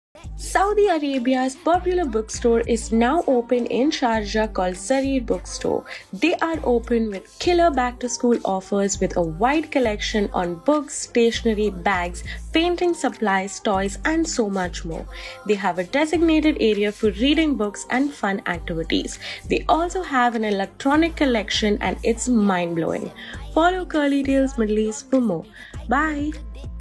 Saudi Arabia’s Popular Book Store Is Now Open In Sharjah | Curly Tales ME
CTDiscovery - Saudi Arabia’s popular book store is now open in Sharjah 😍😍🤩

📍Jarir Book Store - University city - Sharjah

They are open with killer Back to school offers

With a wide collection of books, stationery, bags, paints, toys and so much more

A designated area for reading books and activities

Their electronics collection is mind blowing

Follow CurlyTales ME for more!